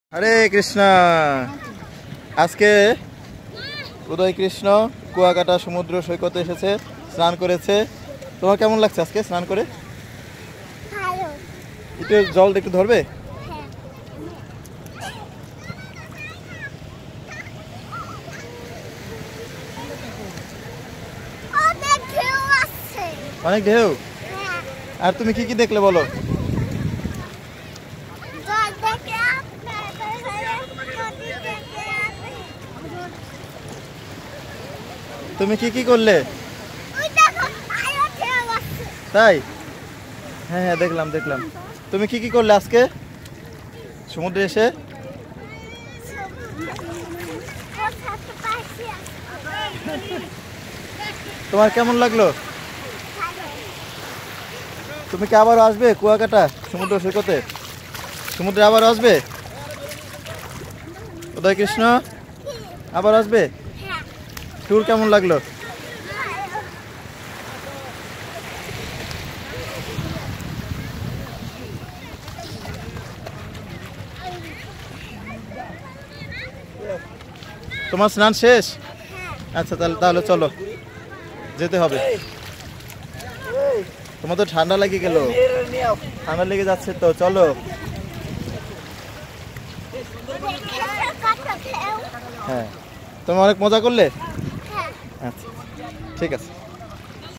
উদয় কৃষ্ণের উপলব্ধি- কুয়াকাটা সমুদ্র সৈকত।ইসকন স্বামীবাগ গীতা একাডেমী কর্তৃক পারমার্থিক শিক্ষা সফর
উদয় কৃষ্ণের উপলব্ধি- কুয়াকাটা সমুদ্র সৈকত।ইসকন স্বামীবাগ গীতা একাডেমী কর্তৃক পারমার্থিক শিক্ষা সফর ২৫ ডিসেম্বর ২০২২হরে কৃষ্ণ 🙏